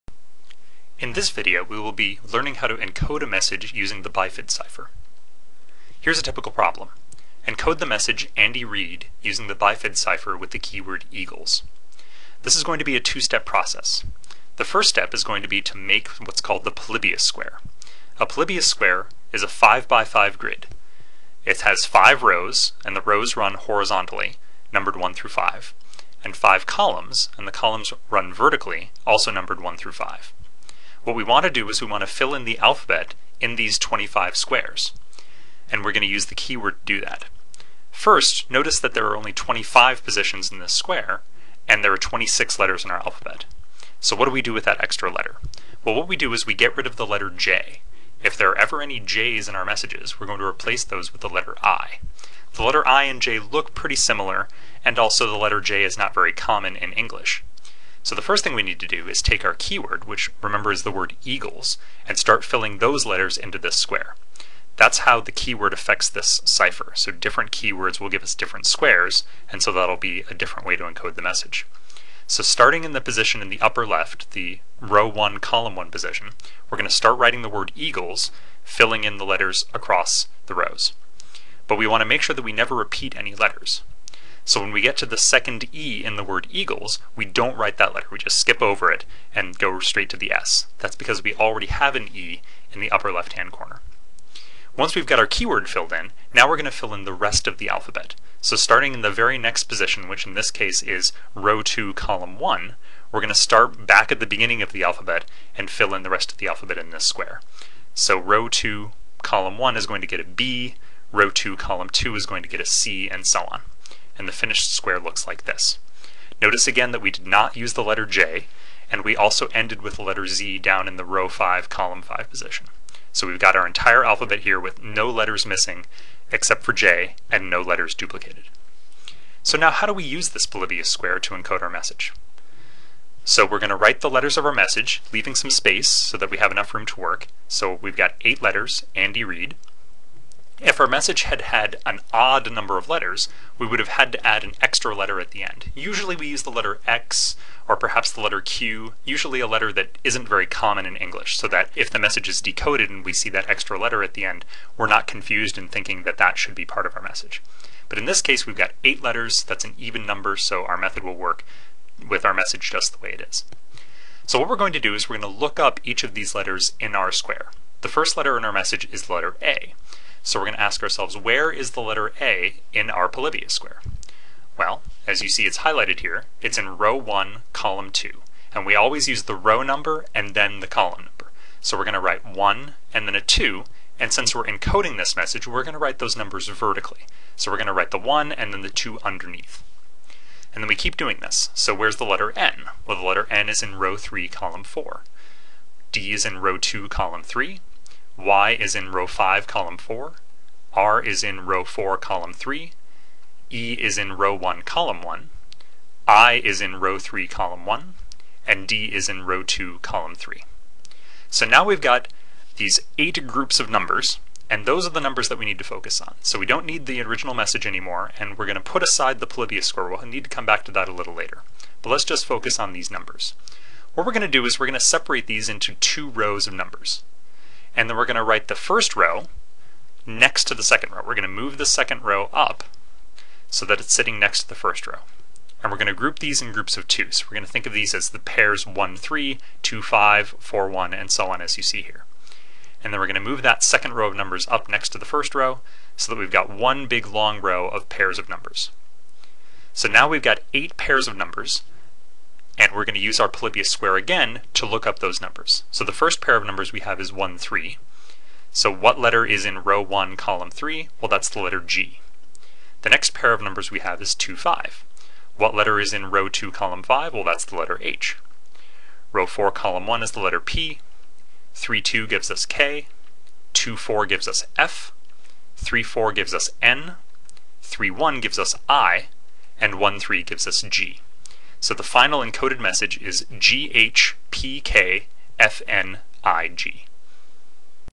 Math for Liberal Studies: Encoding a Message Using the Bifid Cipher
In this video, we work through an example encoding a message using the bifid cipher.